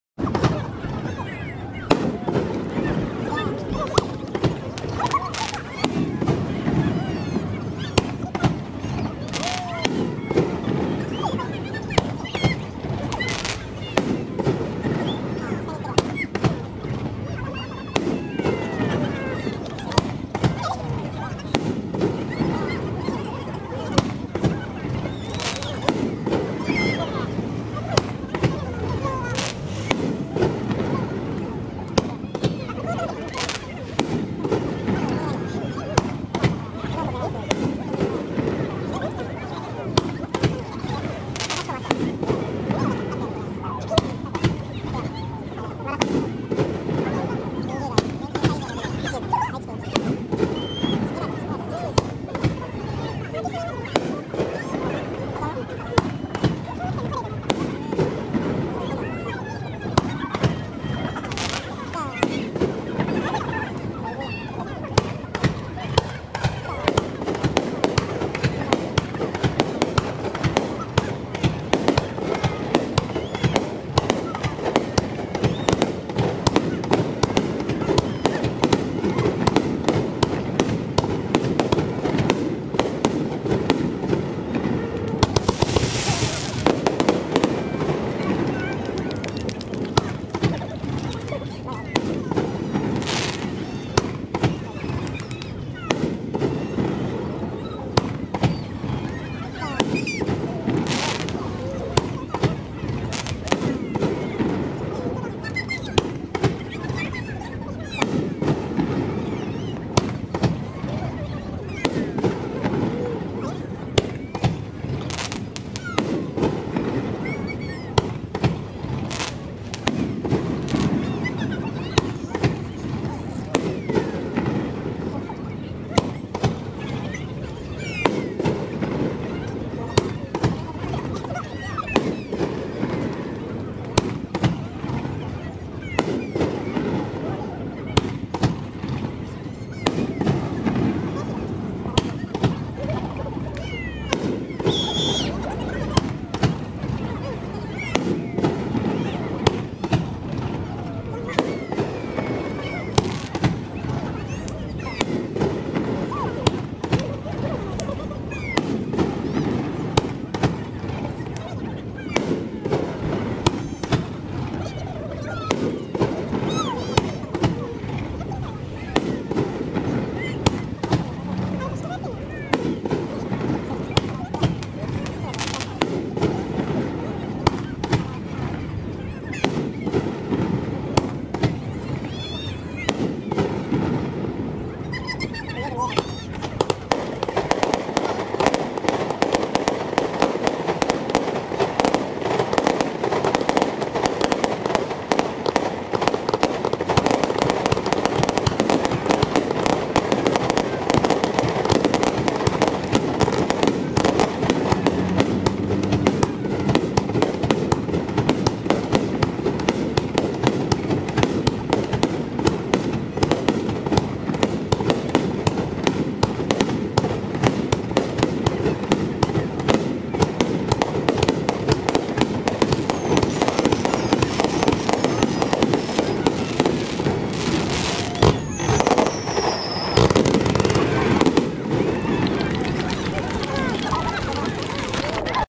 2023年日本ライン夏まつり
コロナ明けの地元の花火大会が8/1～8/10まで木曽川の真中で実施される。
午後8時開始だが、直前まで岐阜県側の上空では高い頻度で「イナビカリ」が走っていた。また、ドローンも飛んでいた(後半に確認できます)。ドローンは禁止のはずではなかったか。